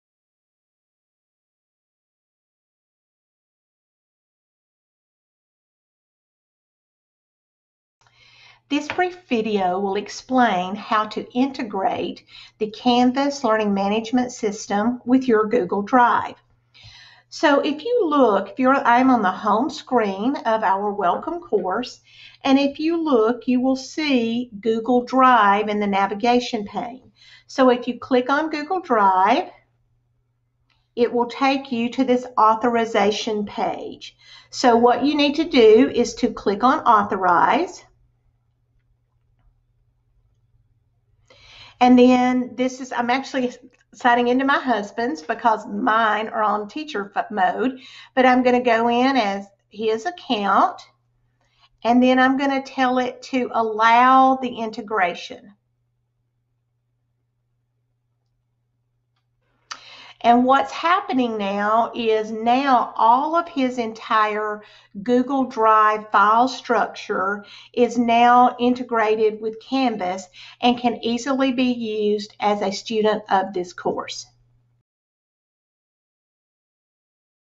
How to Integrate Canvas and Google Drive
Video to support Module 0 for integrating Canvas and Google Drive